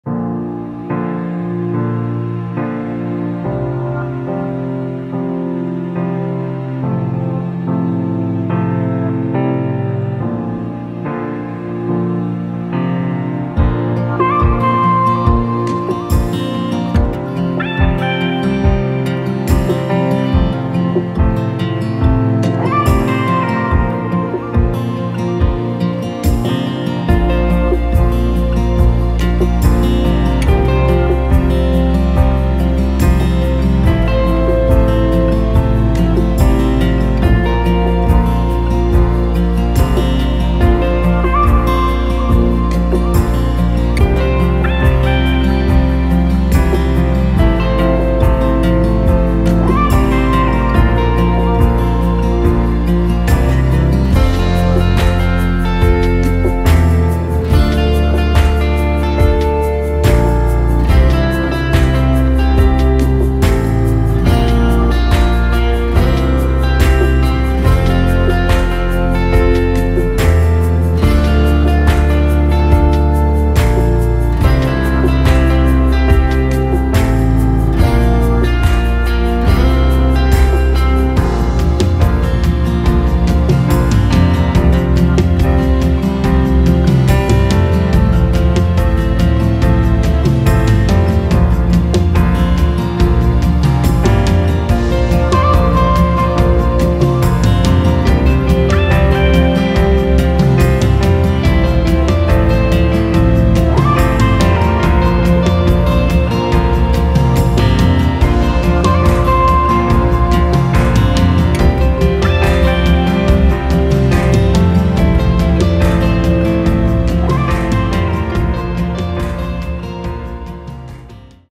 BEHIND THE SCENES WITH BRIANNA | MODEL PHOTO SHOOT
A unique behind the scenes shoot with Brianna! This video with shoot with both a Canon EOS R @ 24FPS and a 25 year old Apitek Fidelity 3100 camcorder (QVGA 320x240 pixel 10FPS) attached to my DJI Ronin RS2. The combination of the modern and retro footage made this video so captivating. Enjoy! videography behindthescenes livinglife model modelshoot retro oldandnew lowtech photoshoot photography beauty beautiful @blessedholisticlife